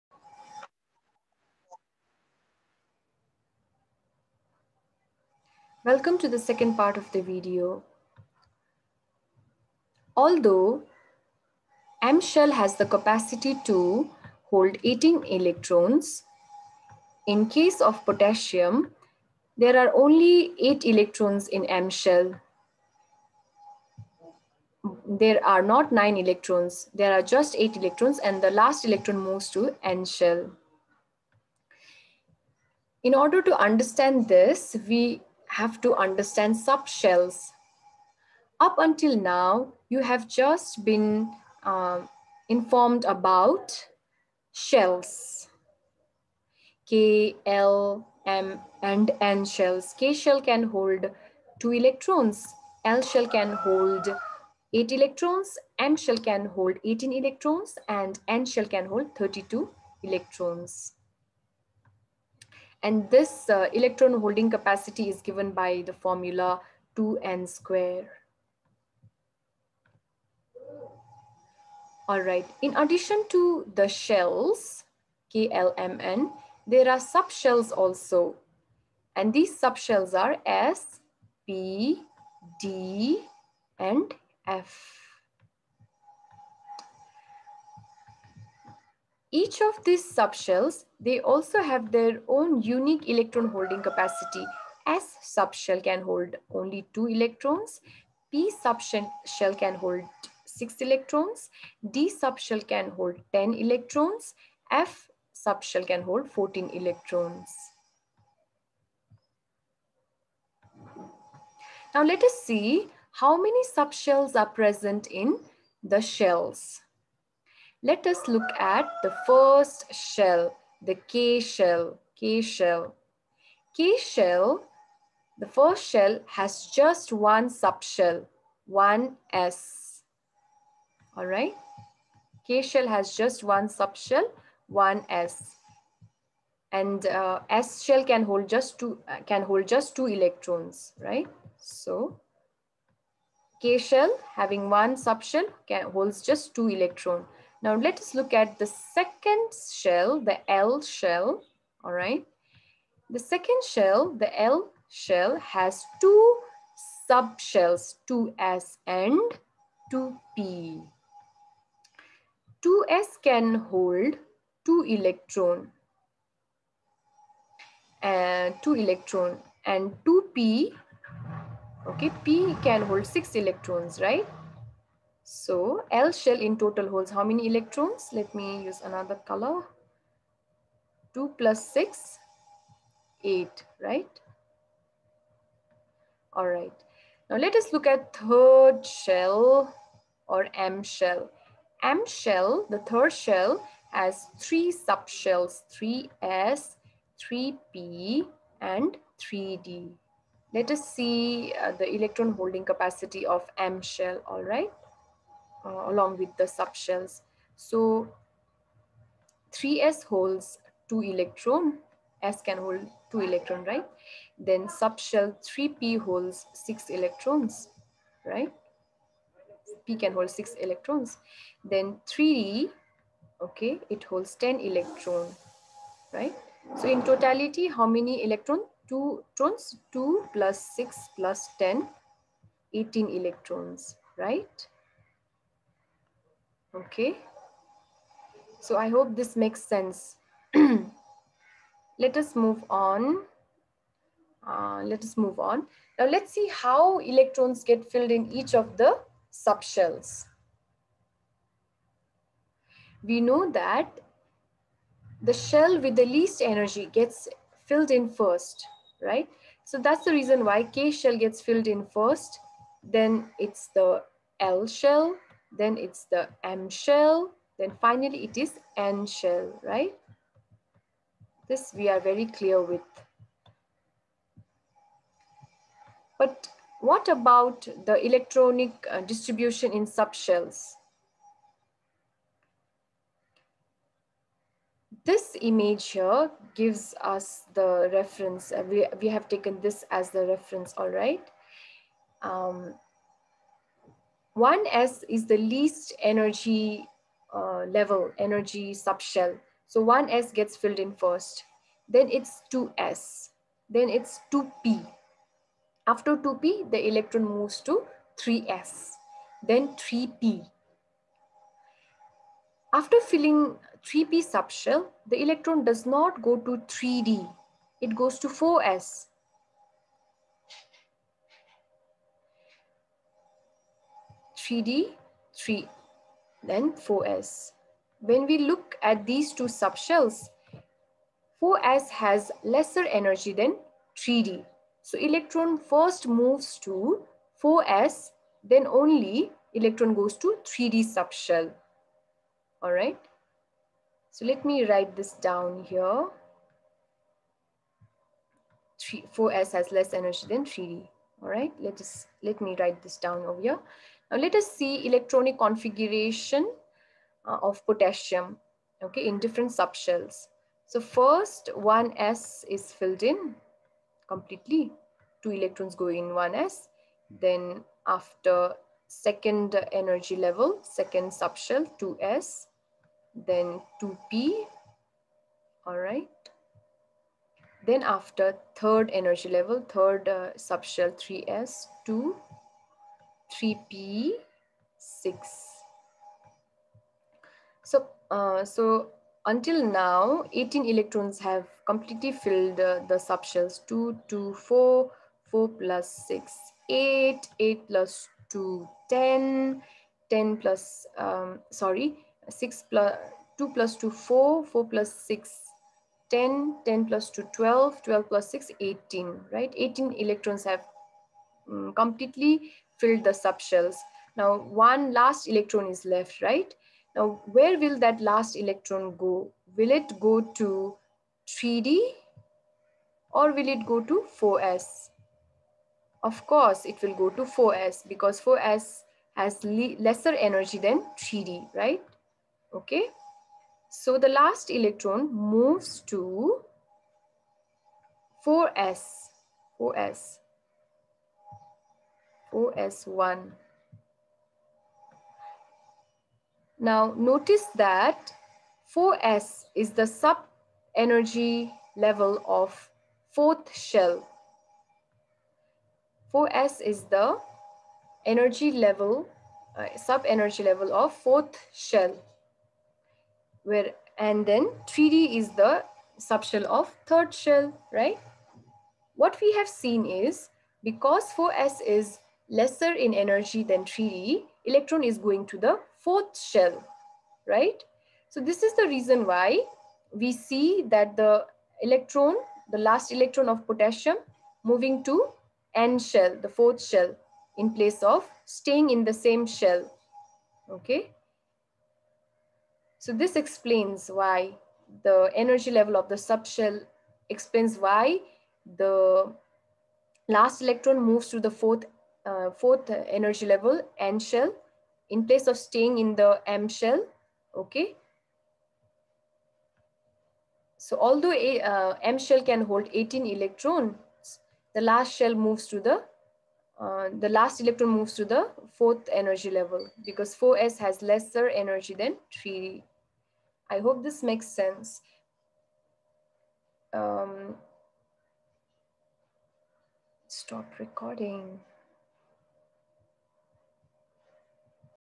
Electronic Configuration: Why M shell of Potassium has just 8 electrons instead of 9 electrons????
M shell has the capacity to hold 18 electrons. However, in Potassium (K), the M shell has only 8 electrons instead of 9 electrons, the last electron moves into the 4th energy level. This can be explained by the concept of sub-shells. 4s has lesser energy than 3d, hence the last electron of K moves to the 4th energy level first and that is why M (3rd shell) has just 8 electrons and the last electron moves to the 4th shell.